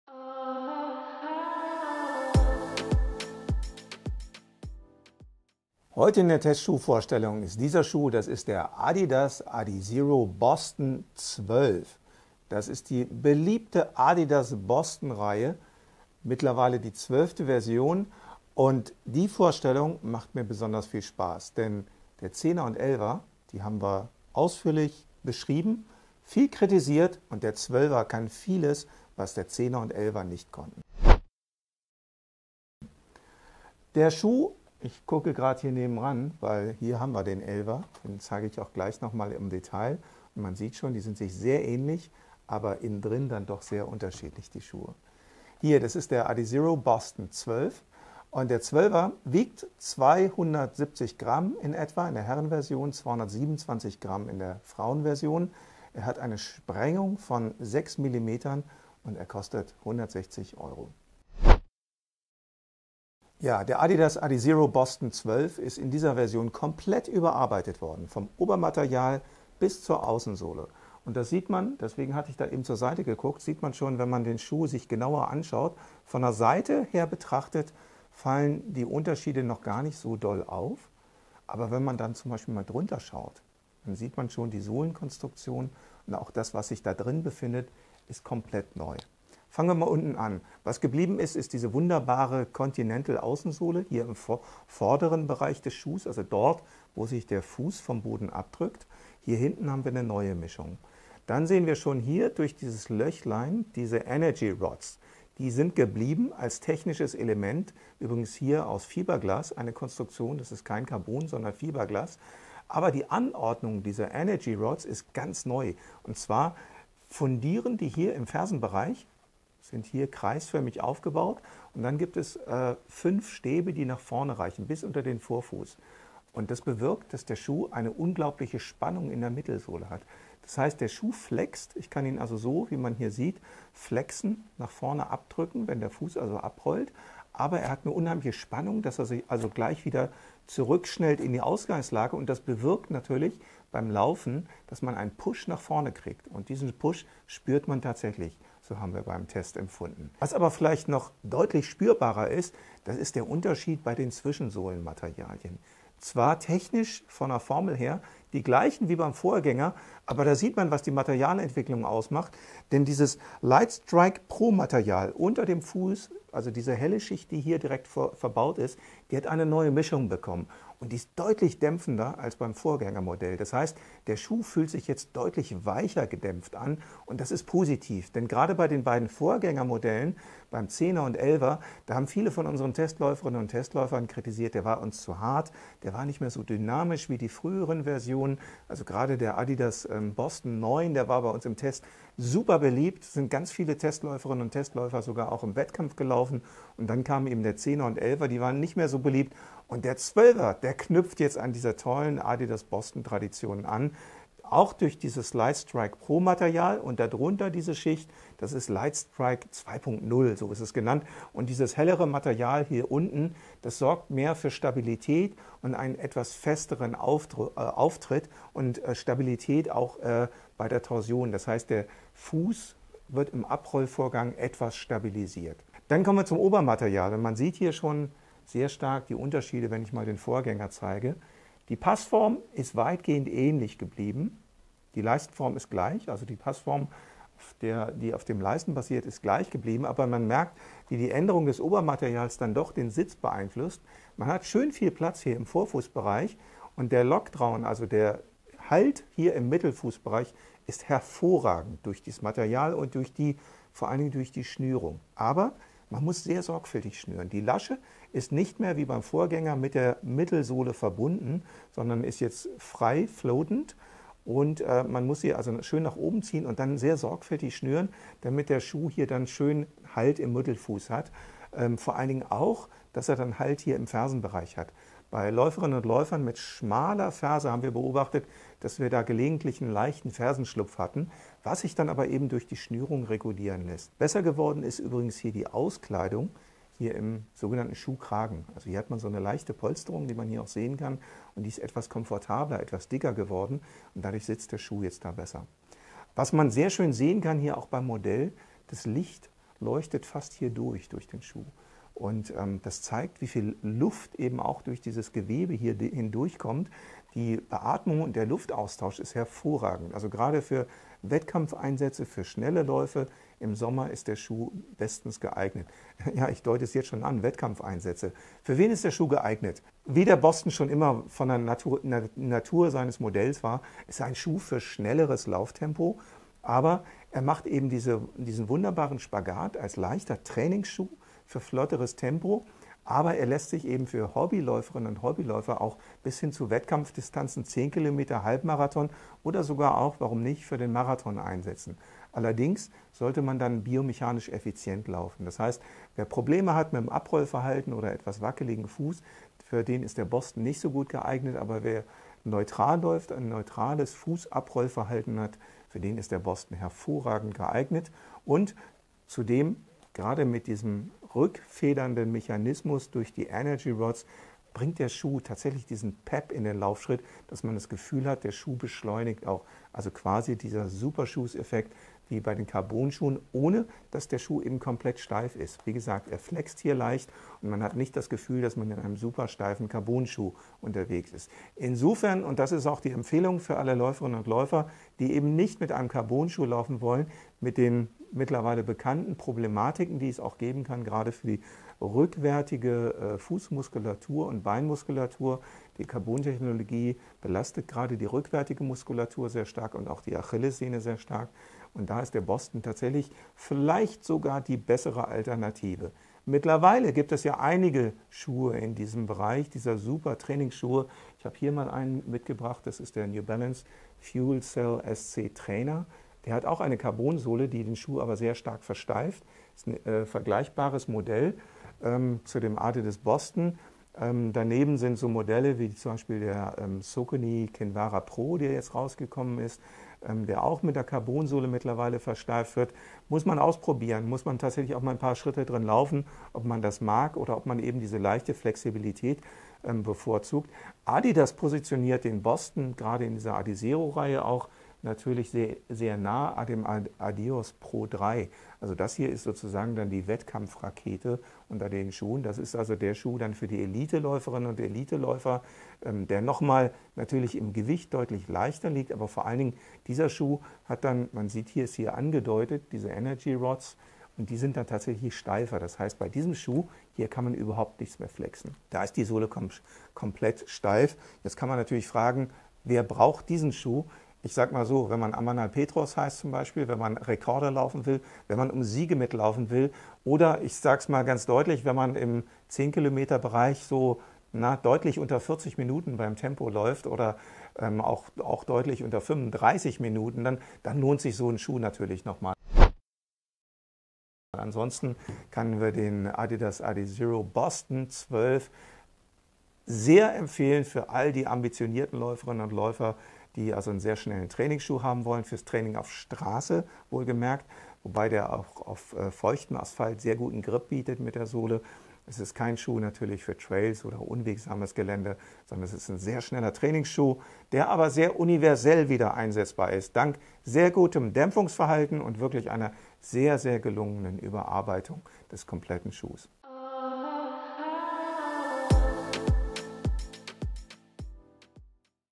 Adidas Adizero Boston 12 im Test
Der Adidas Boston hat uns und vielen treuen Fans in den letzten beiden Versionen nicht ganz so gut gefallen. Ob die 12. Edition wieder das Zeug zum Lieblingsschuh hat, verraten wir in diesem Video.